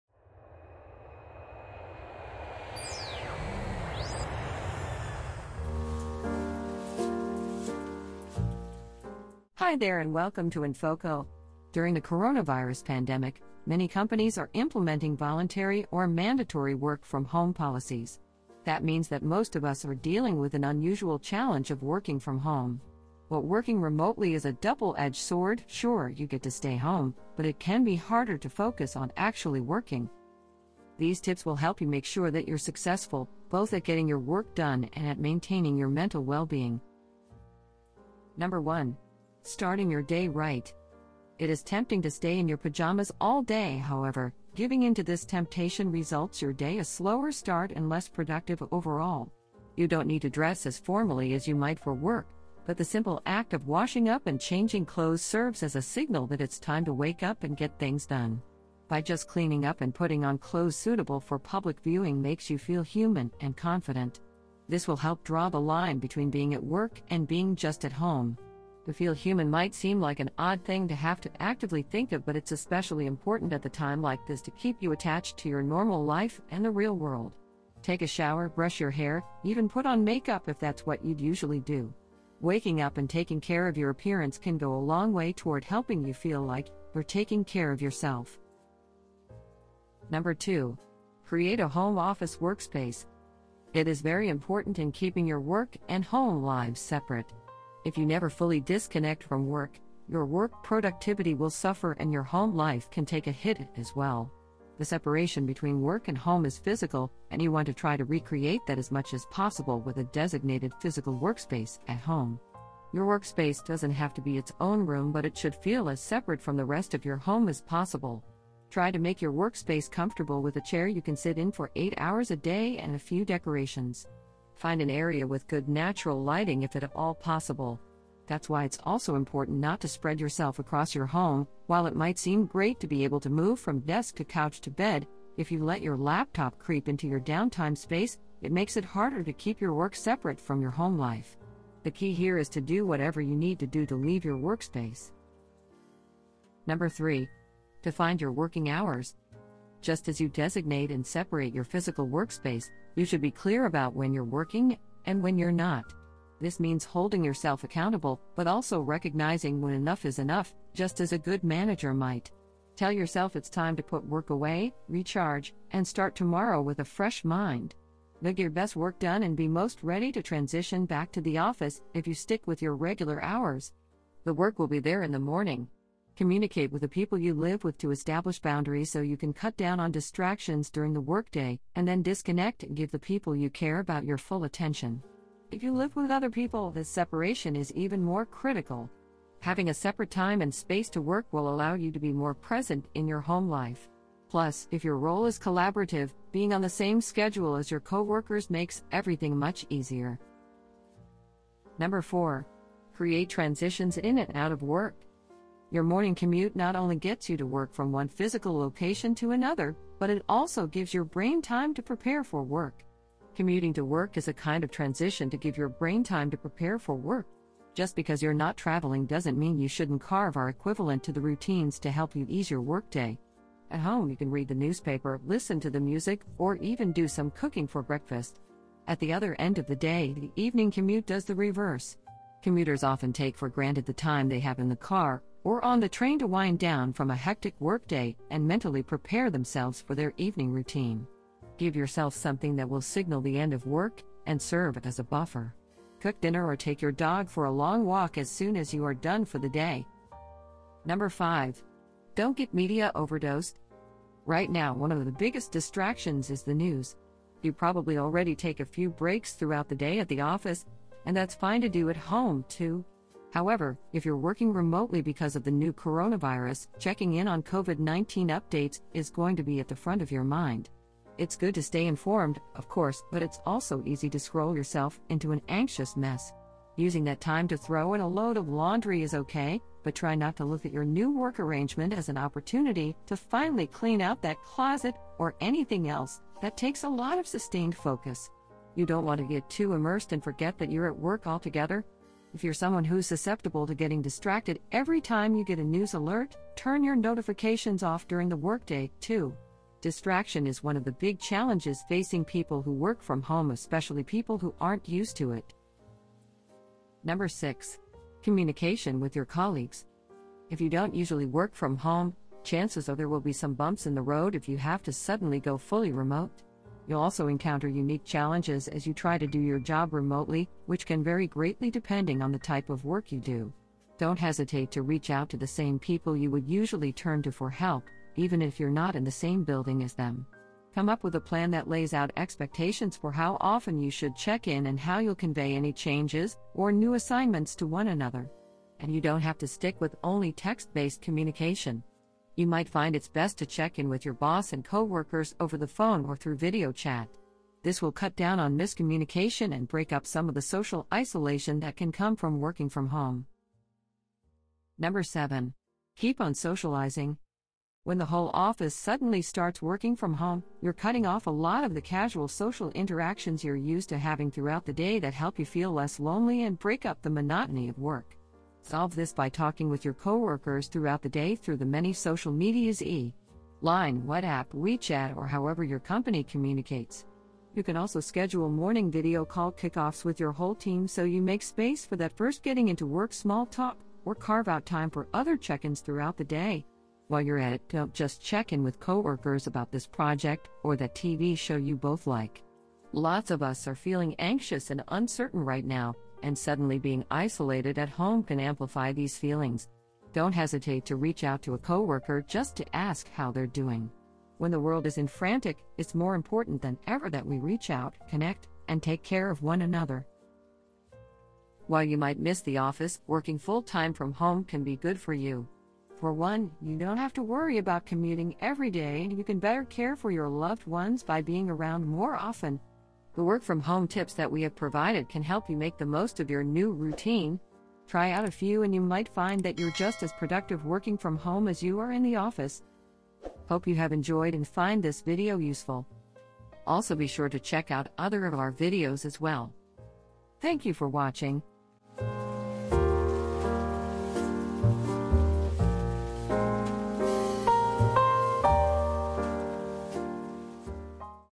Tips for Working from Home During the Covid Pandemic - 7 Guides
In this video you will find 7 tips for working from home during the coronavirus pandemic. During the coronavirus pandemic, many companies are implementing voluntary or mandatory work-from-home policies. That means that most of us are dealing with an unusual challenge of working from home. But working remotely is a double-edge sword — sure, you get to stay home, but it can be harder to focus on actually working.

Also be sure to check out other of our videos as well.